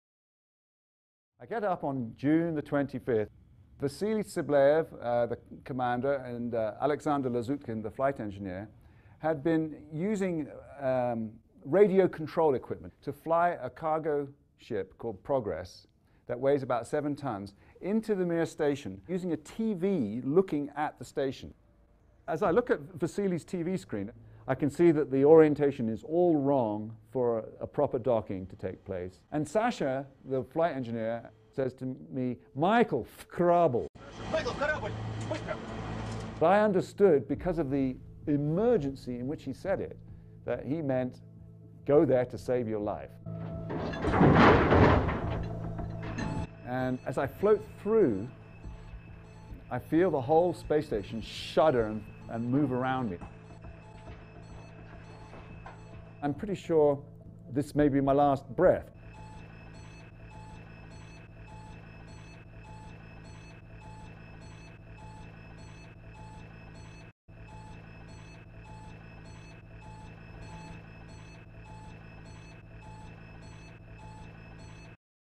'SpaceTalking with an Astronaut' - Astronaut Michael Foale - Mir Space Collision
Find out what really happened on the Mir Space Station this Thursday with astronaut Michael Foale when he retells his story. 'Keeping your Cool in a Space Disaster' in this weeks 'Space Talking with an Astronaut'

Michael Foale is a British born hall of fame NASA astronaut, he saved the Hubble Space Telescope and was on board the Mir Space Station when a Russian Soyuz spaceship crashed into the station causing the station to lose power and air, tumbling out of control in space. Tune in this Thursday and find out how Mike and his crew revived the Space Station on the brink of unthinkable disaster and managed survive one of the most hostile environments known to man. All in this weeks episode of ‘SpaceTalking with an Astronaut’